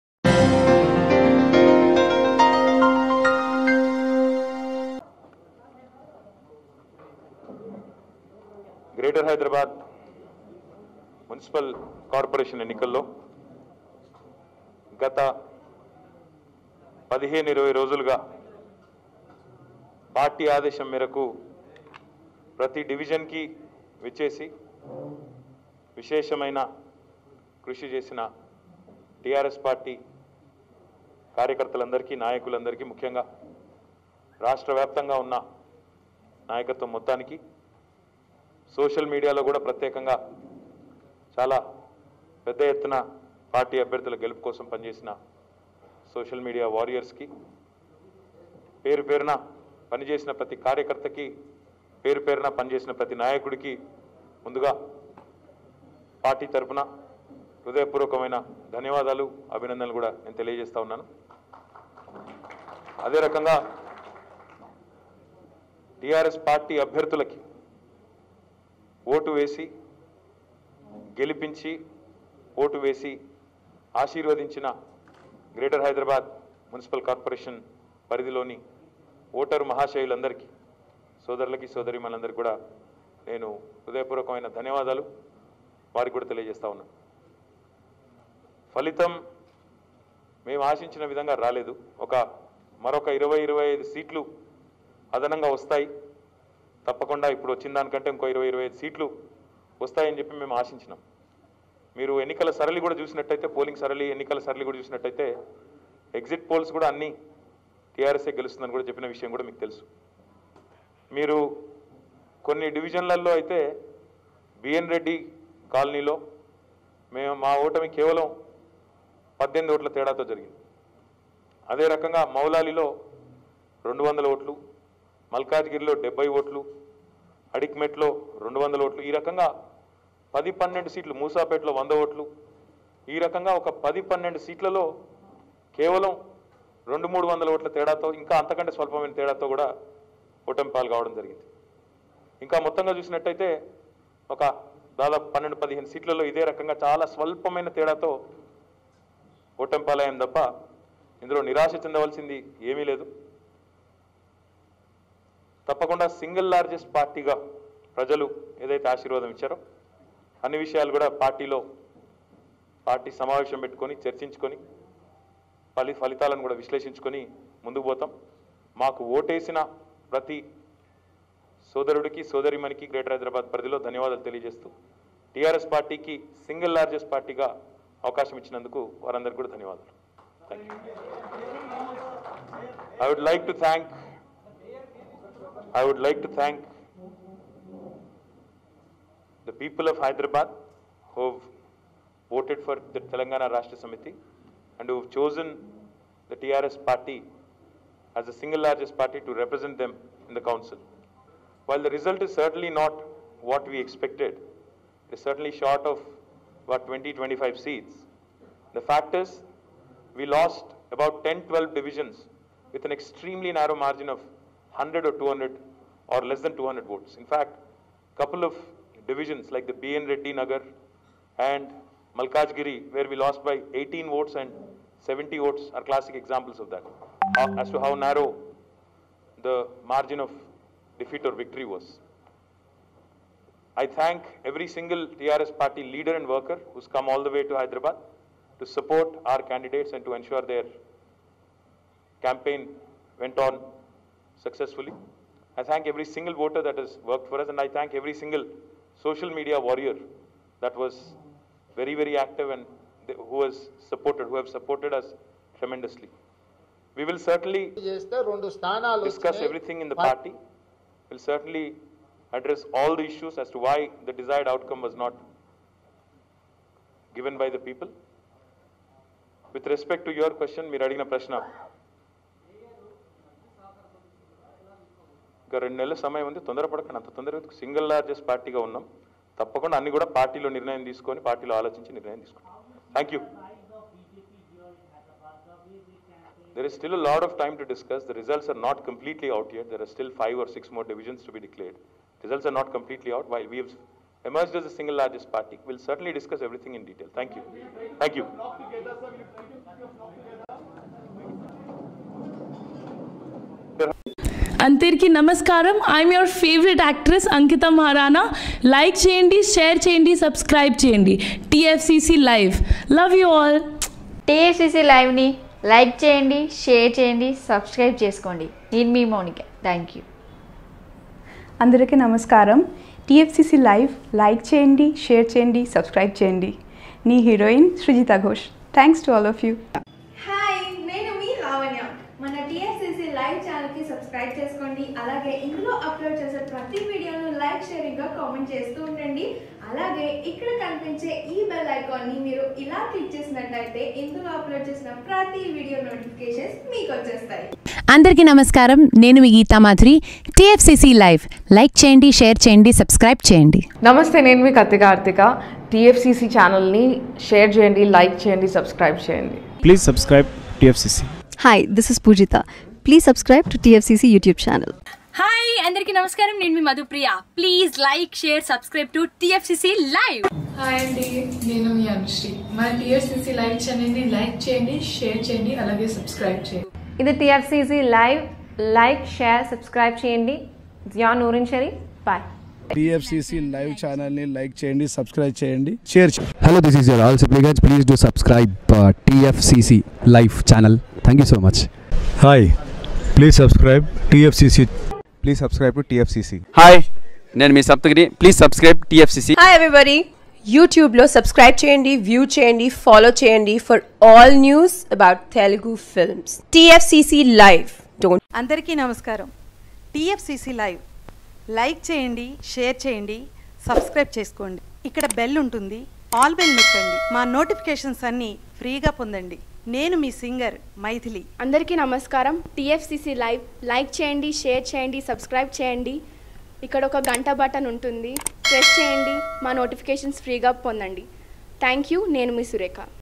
Minister KTR About GHMC Election Results 2020 | Telangana | TFCCLIVE
Minister KTR Says Thanks To Hyderabad People Over GHMC Elections Results